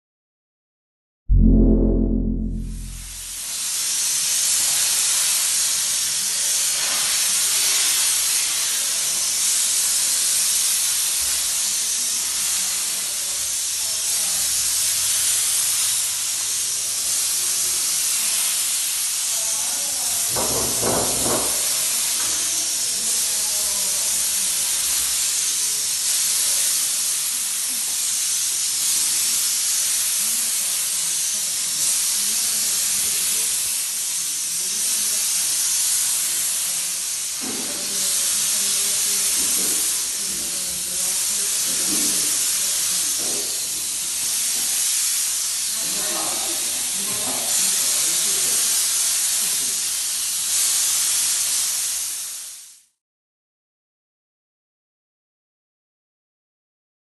Sanitizing professional studios with steam | Sanitizzazione Ufficio professionali con il vapore
Sanitizzazione di Ambienti professionali con il vapore fino a 190°: un metodo Sicuro e completo per la pulizia profonda, l'igienizzazione, la sterilizzazione e la sanificazione totale

Sanitization of professional environments with steam up to 190 °: a safe and complete method for deep cleaning, sanitation, sterilization and total sanitization